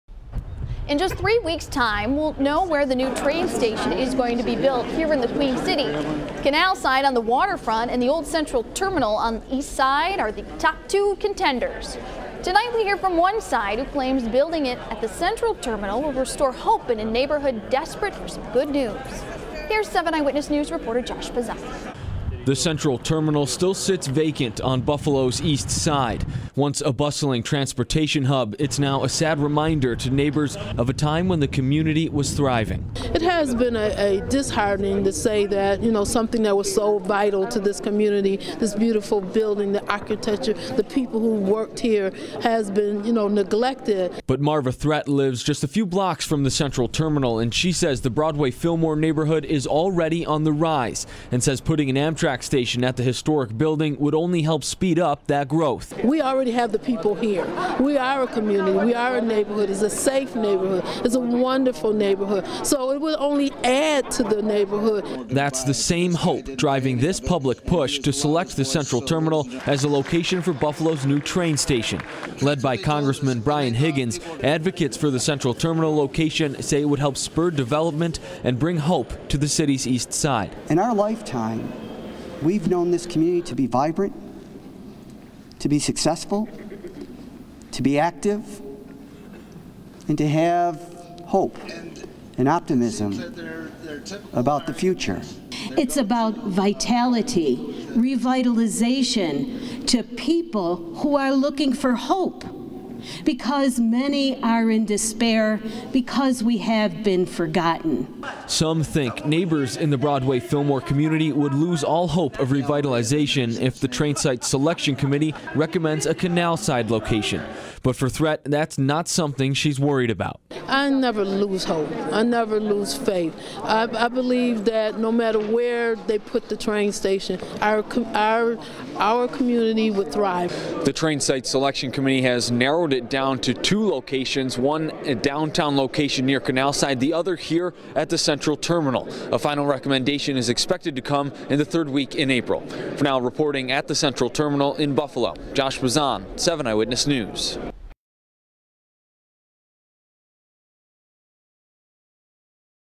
"Hope" drives the push to put Buffalo's train station at Central Terminal
The Central Terminal was once a bustling transportation hub.  Now, it sits empty and reminds East Side neighbors of a time when their community was thriving.